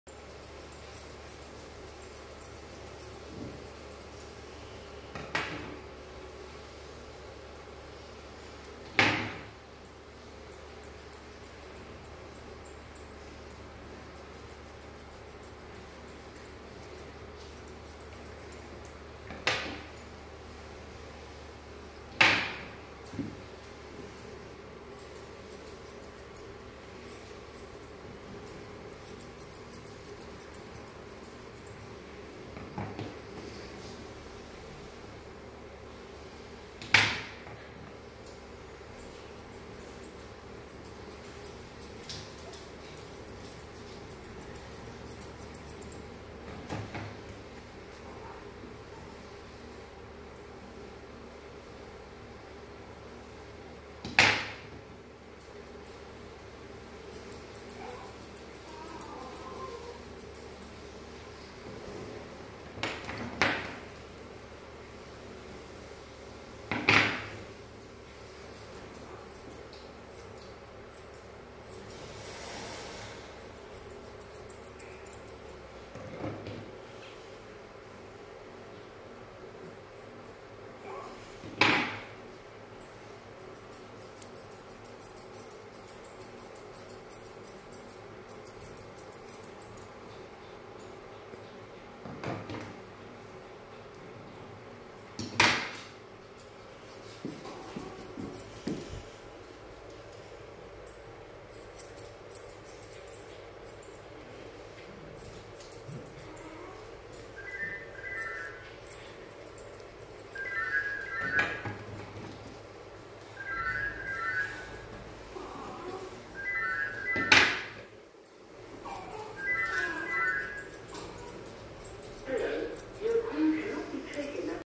Gizmos groom by Groomers on the Green
Demomstration on grooming a bichon head to get that perfect round shape using scissors and thinners